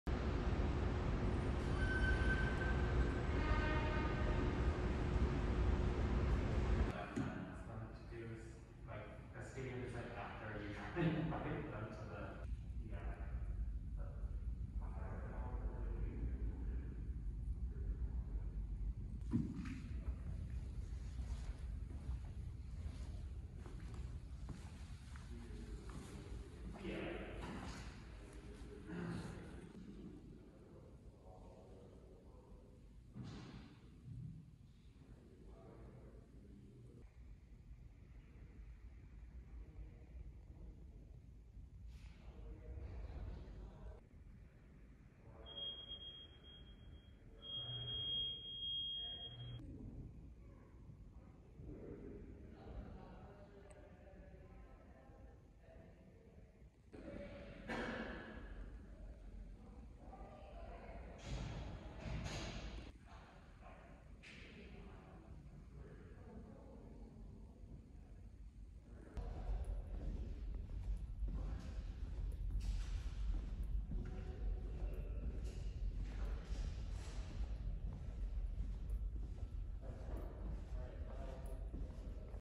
Andrew Kreps Gallery: Fredrik Værslev - World Paintings (NY Gallery Vlog) [4K]
Andrew Kreps Gallery: Fredrik Værslev - World Paintings (NY Gallery Vlog)

Dates: November 13 - December 19, 2020